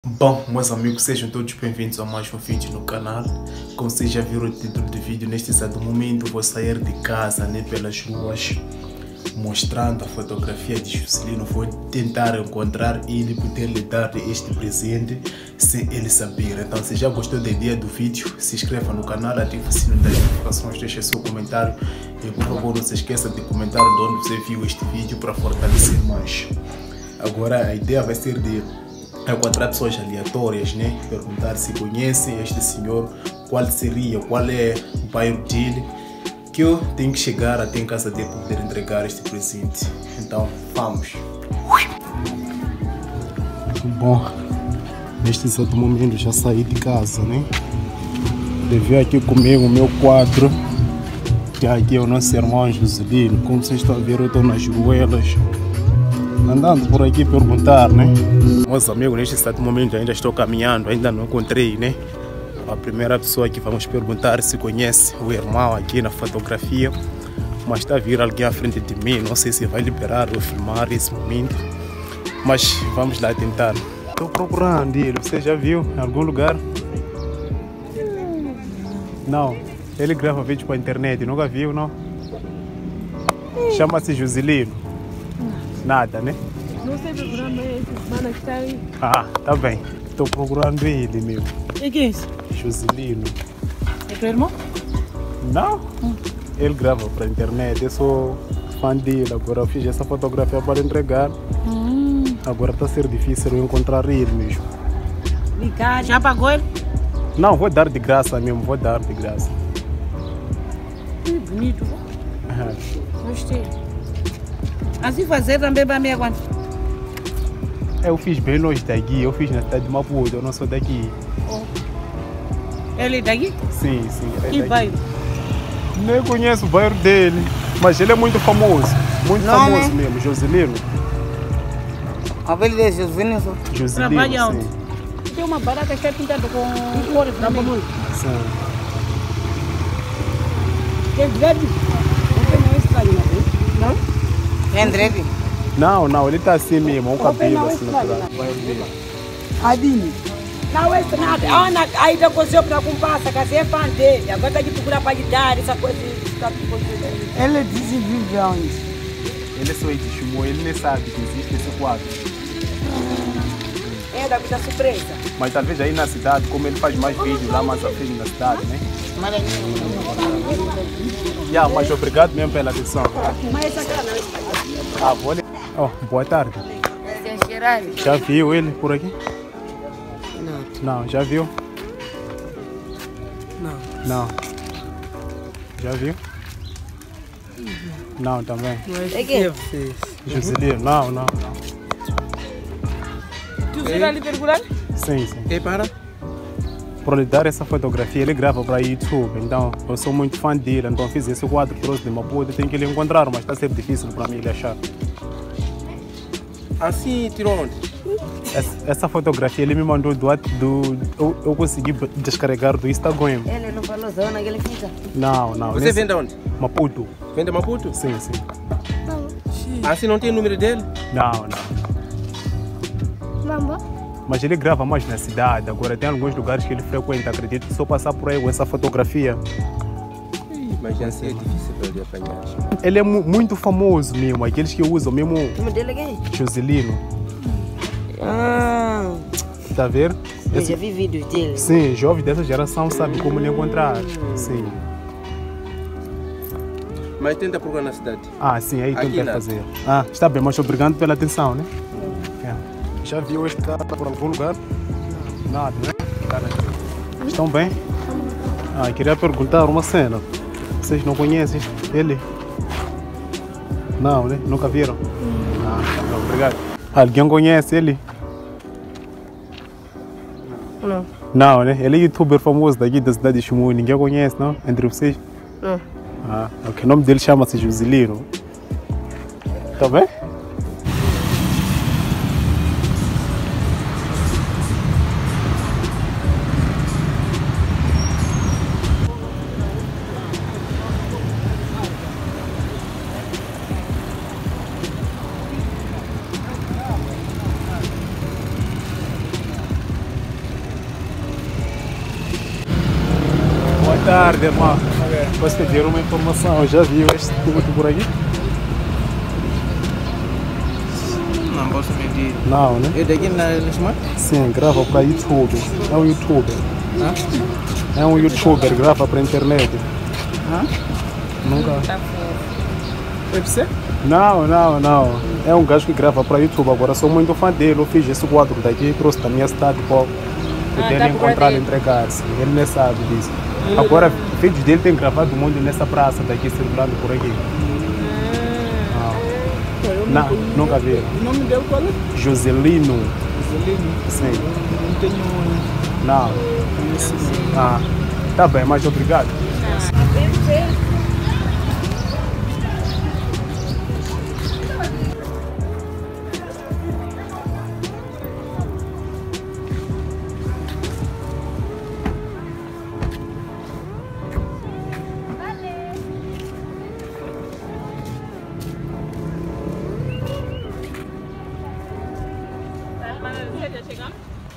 Procurei joselino mostrando foto dele nas pessoas, ENCONTREI ele?
Bom neste exato momento vou sair pelas ruas mostrando a fotografia do joselino para pessoas aleatórias para ver se alguém consegue identificar ele e me mostrar em casa de joselino para lhe entregar a fotografia dele